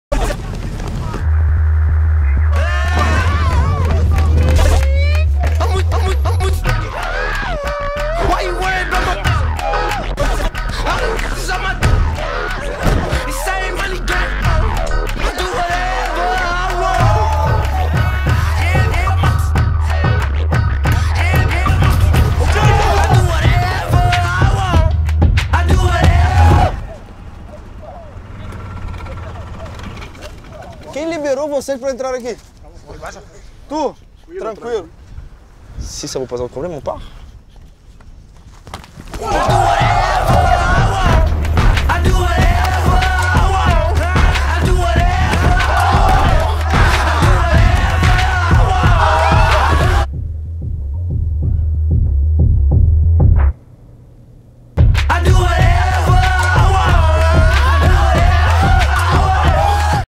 Adidas - Real Madrid y Tang Squad
Zinedine Zidane, Marcelo y el Tango Squad: creatividad al servicio de adidas